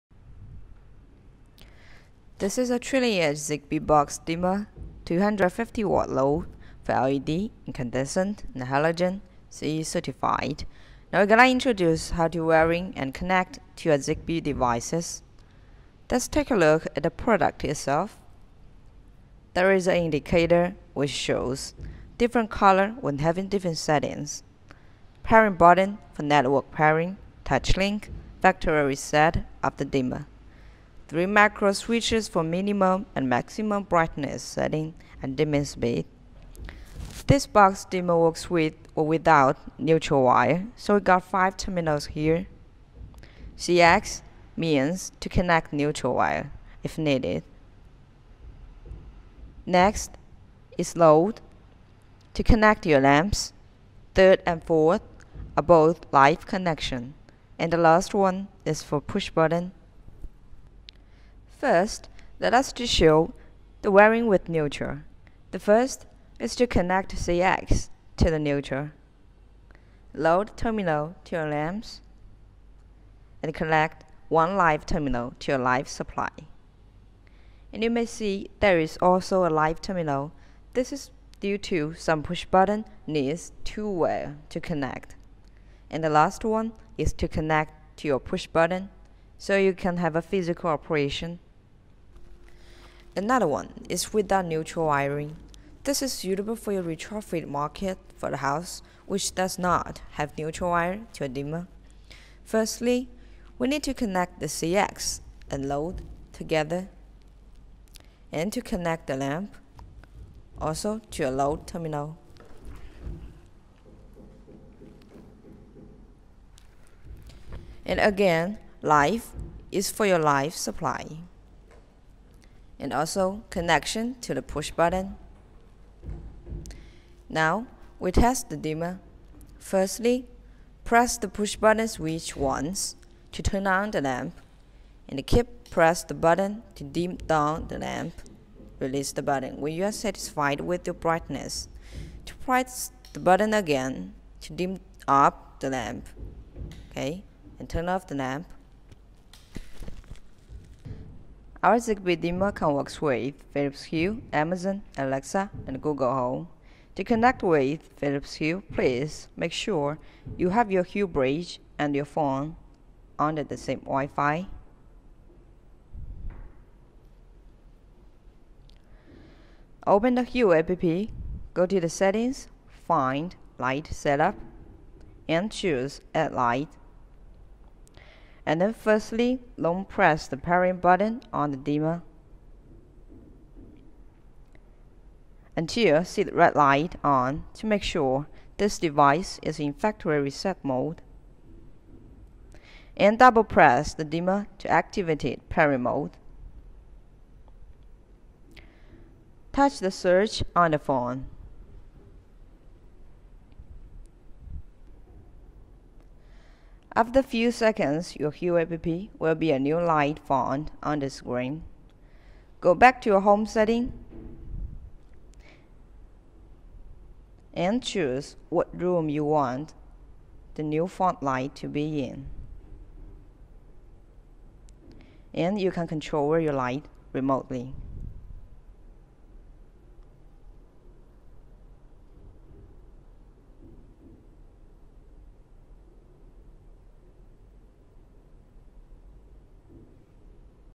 Zigbee 3.0 Push Dimmer work with philips hue no neutral required by HZC Electric - D070K2-ZG
D070K2-ZG is a ZIGBEE device that can be controlled by other ZIGBEE devices to switch and dim the brightness of a connected light.  The dimmer can be included and operated in a ZIGBEE network by any other ZIGBEE certified device and all non-battery operated nodes within the network will act as repeaters to increase the reliability of the ZIGBEE network.

When incorporated into your ZIGBEE system this device is perfect for switching and dimming your mains powered LED lighting and it is also compatible with incandescent mains powered Halogen lights, Low Voltage Halogen lights with electronic transformers and low voltage LED lighting with transformers.

As a high-quality product it is perfect for both commercial and domestic use.